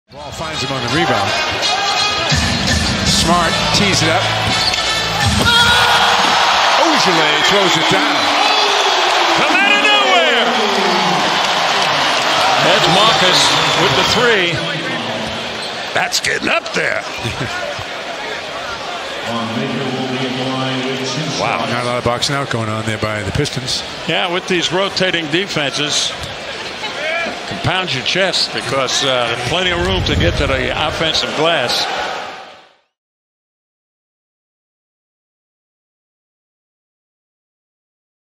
Semi Ojeleye climbs up the ladder for a BIG putback dunk!
Special thanks to Patrons:

► Matthew Berlanti
► Timothy Galebach
► John Kinsella
► Alan Post